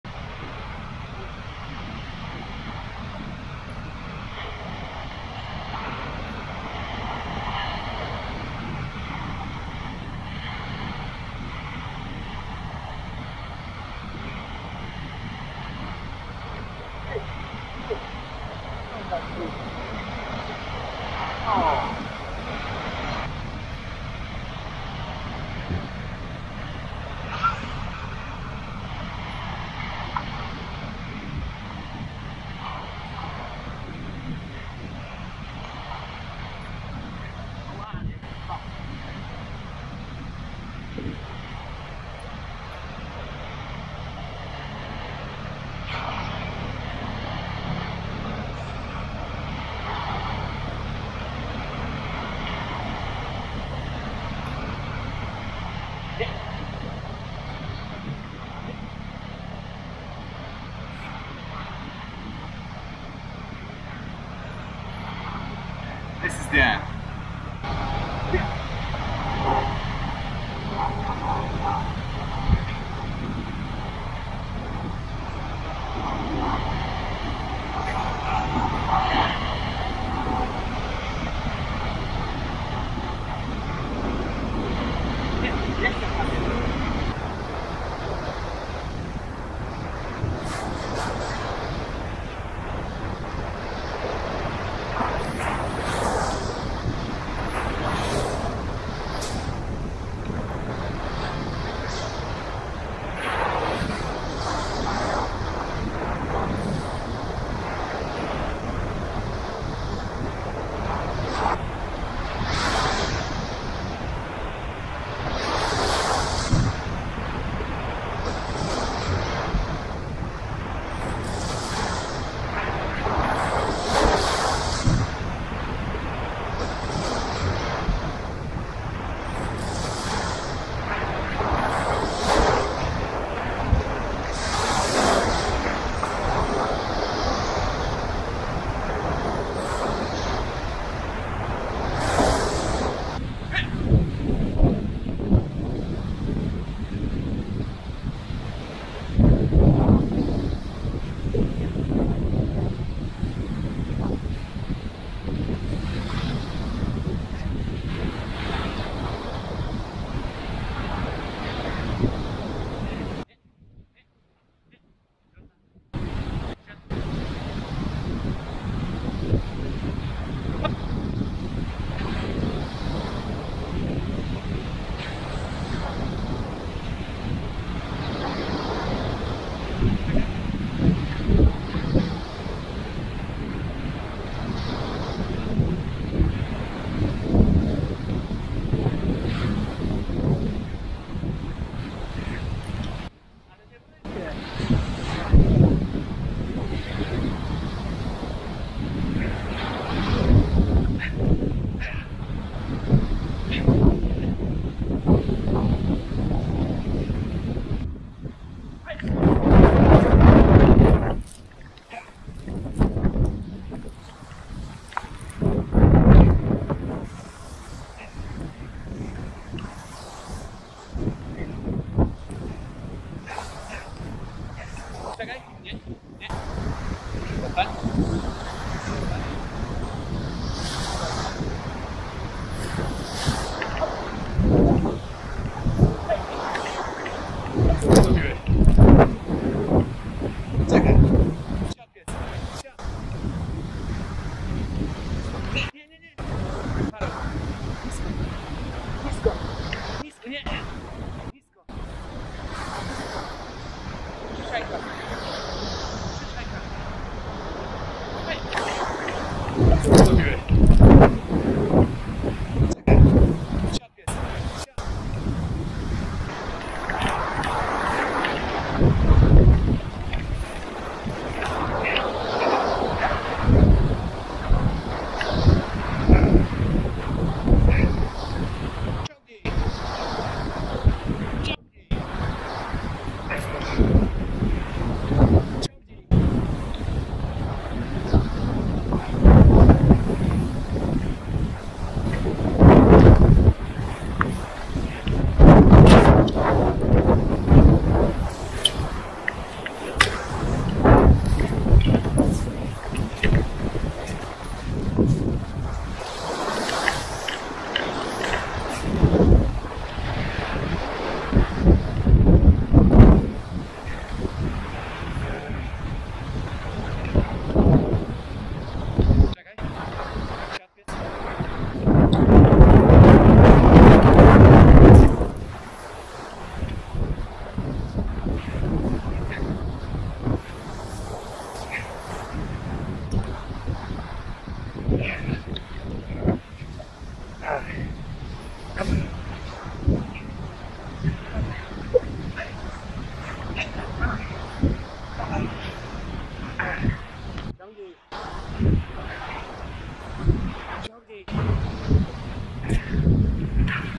Playing with a weimaraner on the beach. Tuggling with dog.
Tuggling with dog is full of fun. Playing with a weimaraner on the beach is energetic. Buy tuger strong enought for your dog 😀